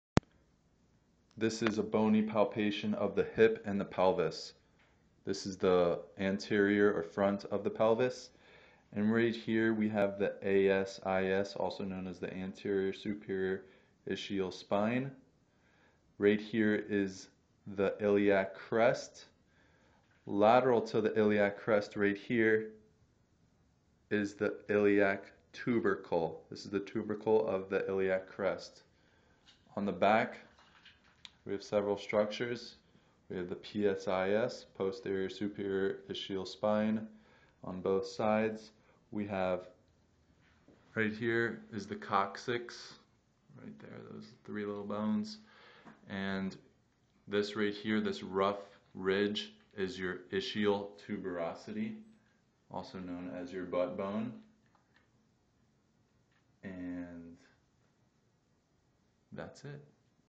Bony Palpation of the Hip and Pelvis
Various structures of the hip and pelvis are covered.